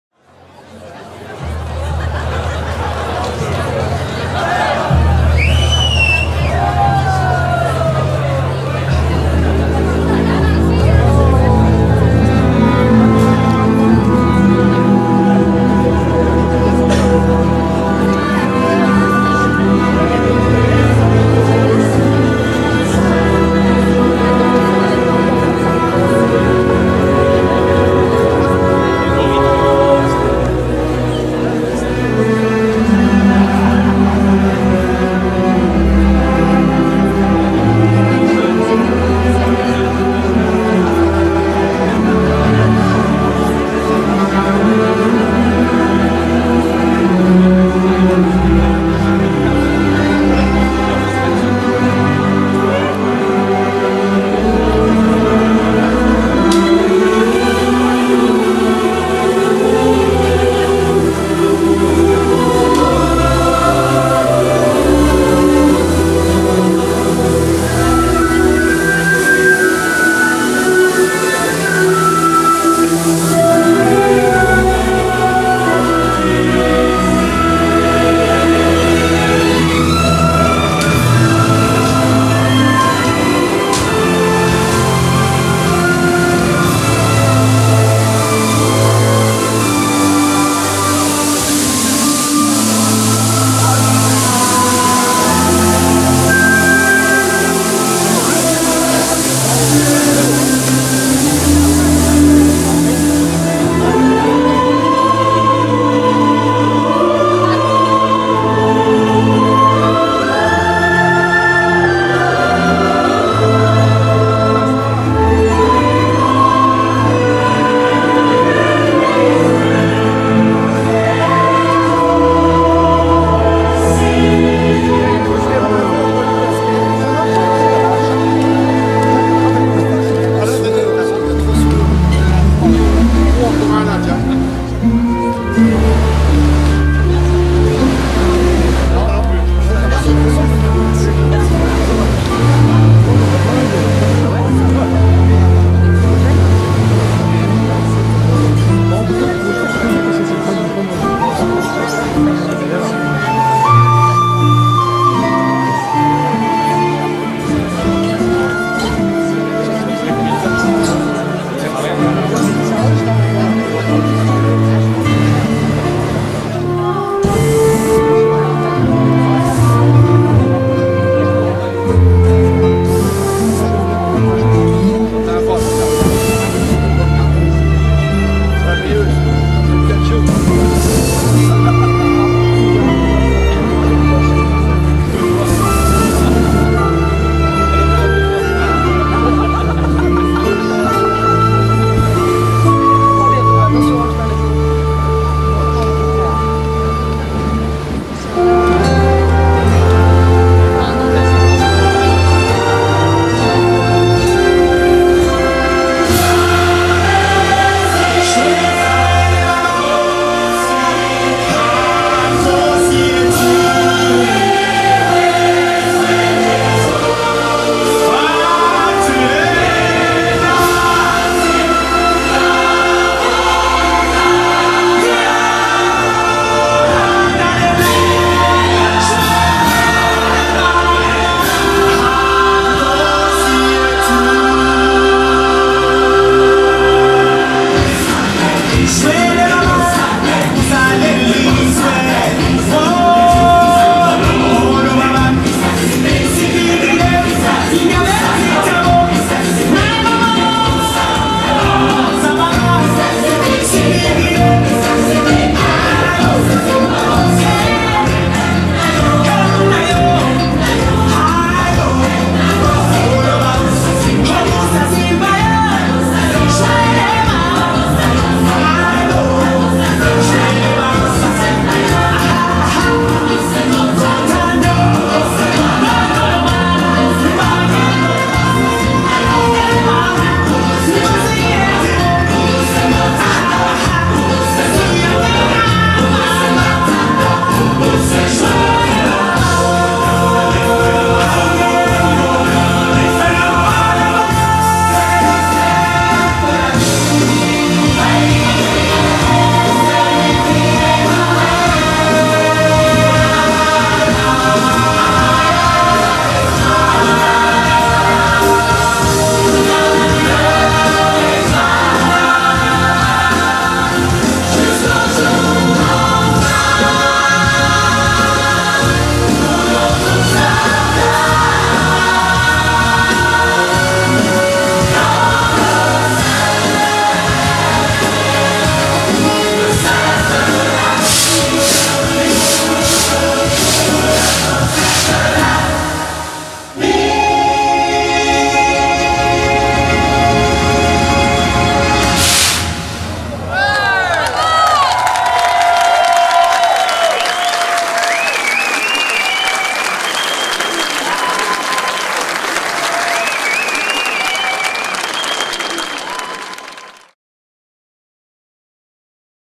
feux d'artifices pour les 30 ans du zoo d'Amnéville
Feux d'artifices froid et non détonnant pour ne pas déranger les animaux.